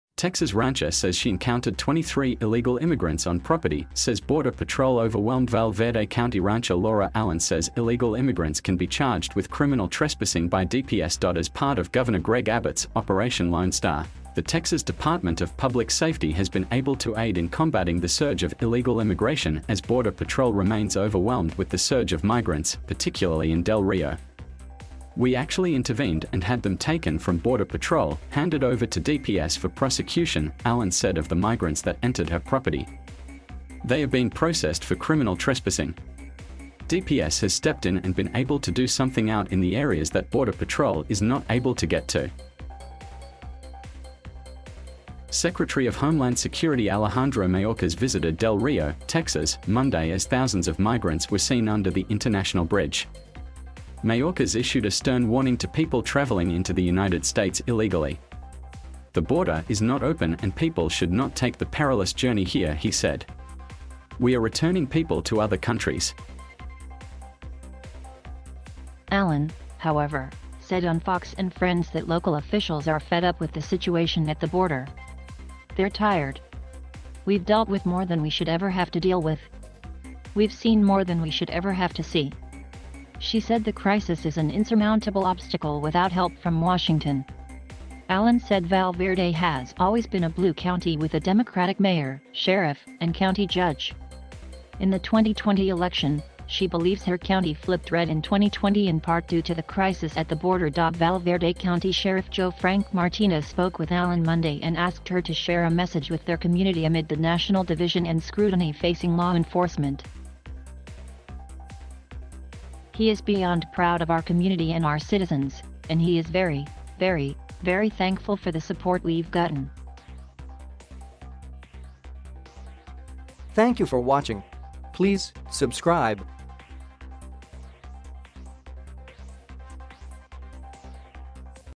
BREAKING NEWS! Texas rancher says she encountered 23 illegal immigrants on property, says Borde
News of the world: BREAKING NEWS! Texas rancher says she encountered 23 illegal immigrants on property, says Borde. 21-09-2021

Texas rancher says she encountered 23 illegal immigrants on property, says Border Patrol overwhelmedVal Verde County rancher Laura Allen says illegal immigrants can be charged with criminal trespassing by DPS.As part of Governor Greg Abbott’s "Operation Lone Star" the Texas Department of Public Safety has been able to aid in combatting the surge of illegal immigration as Border Patrol remains overwhelmed with the surge of migrants, particularly in Del Rio."We actually intervened and had them tak

confusion-1.